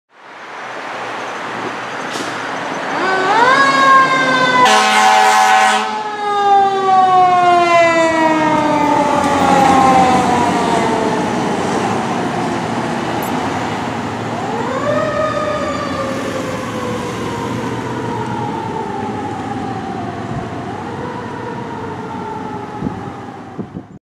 LACoFD Engine 163 Responding
Engine 163 Responding to medical in their first in

Engine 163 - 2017 KME Severe Service

Bell, California - April 2020